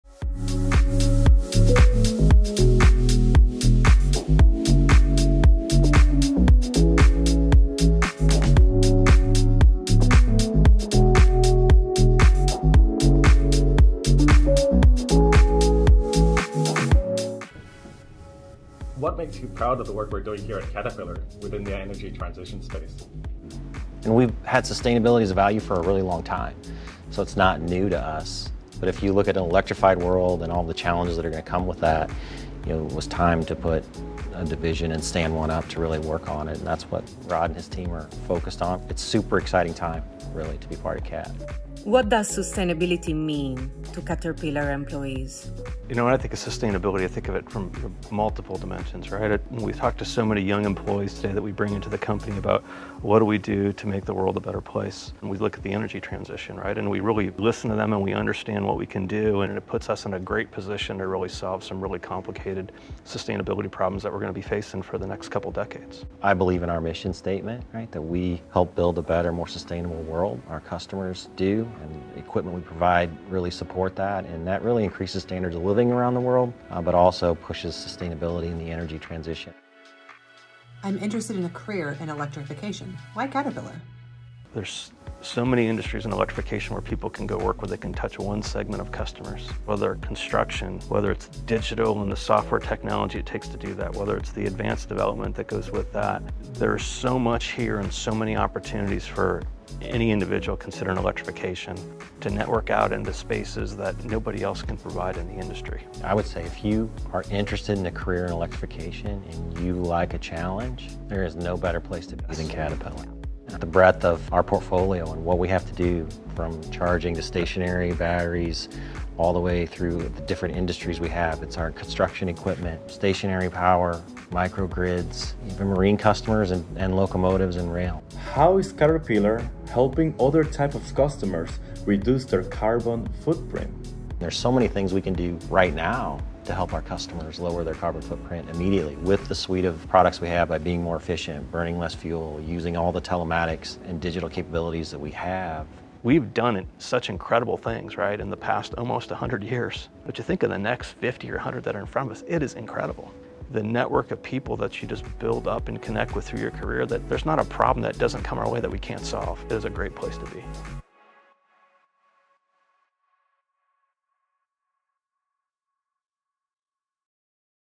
The Future of Caterpillar
Are you interested in a career in electrification? There is no better place to look than TeamCaterpillar. Hear from Joe Creed, Group President of Energy and Transportation, and Rod Shurman, Senior Vice President of Electrification and Advanced Power Solutions how we are playing a major part in the energy transition.